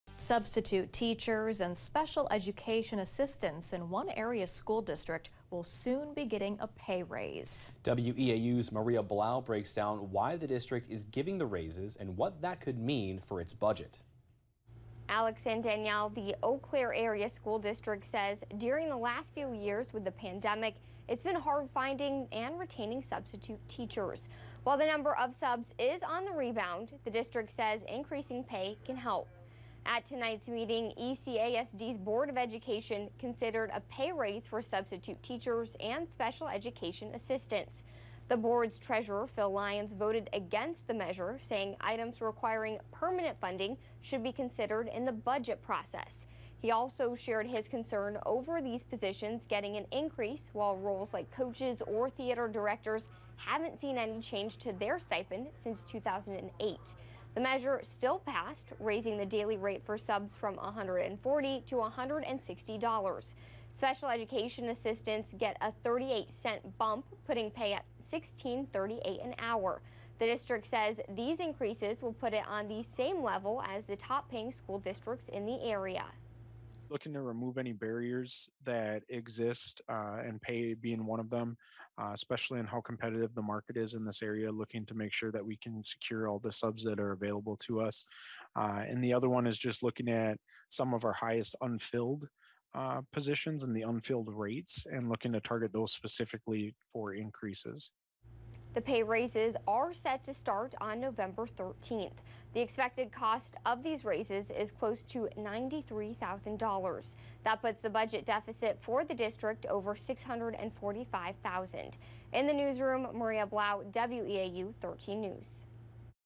ECASD approves pay raise for substitute teachers
The increase is expected to start on Nov. 13.